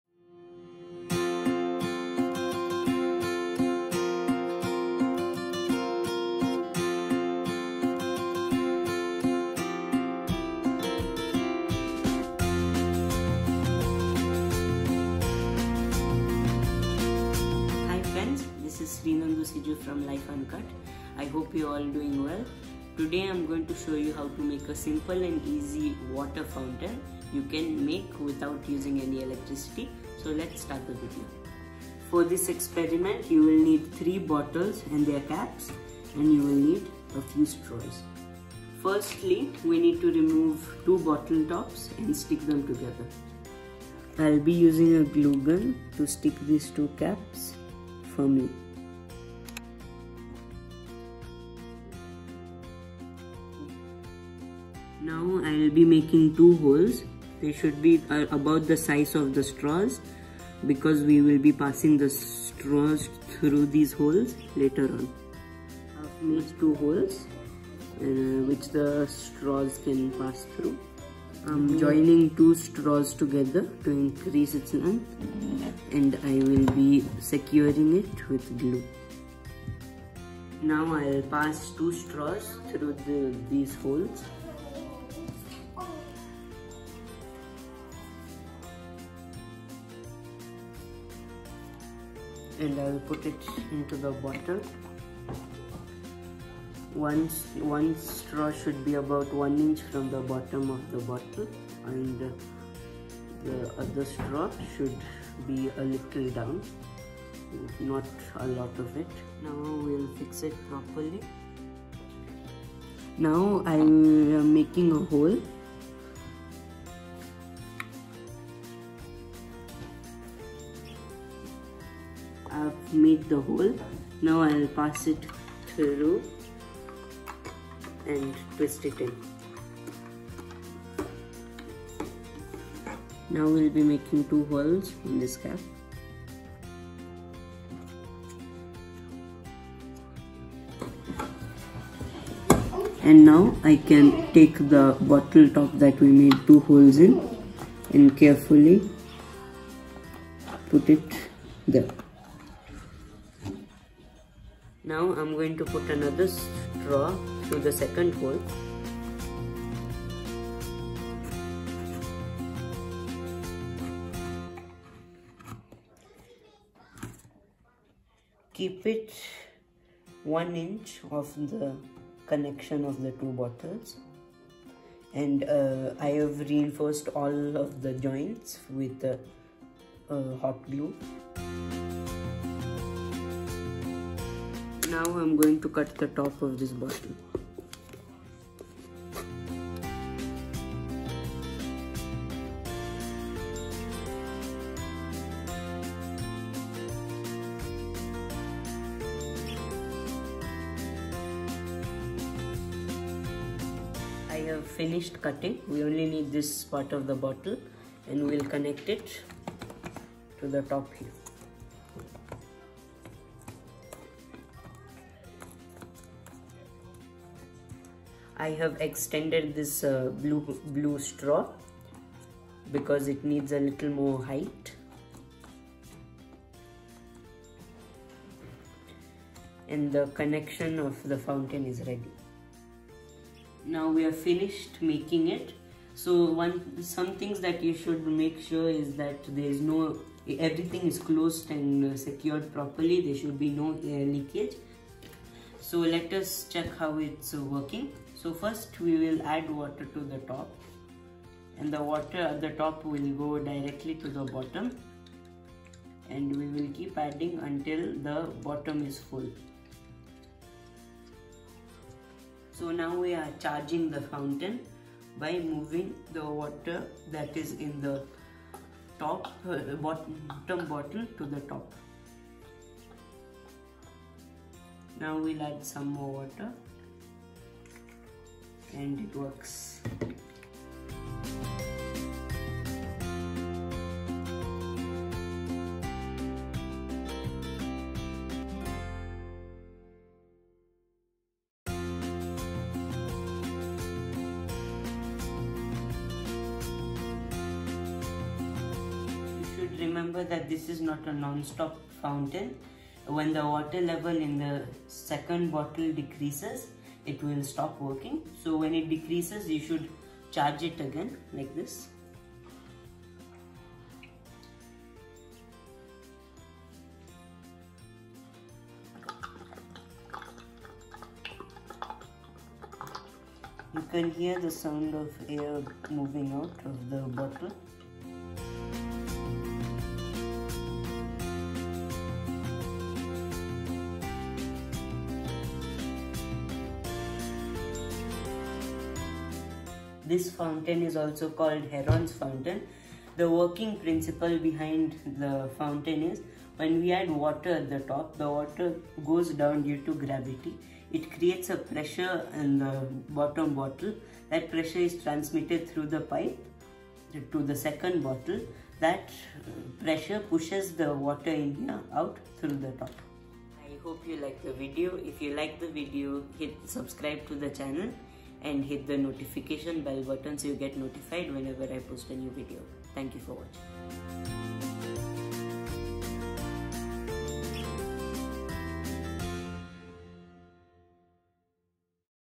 How to make a water fountain that works without electric power ( Heron’s Fountain)
Herons fountain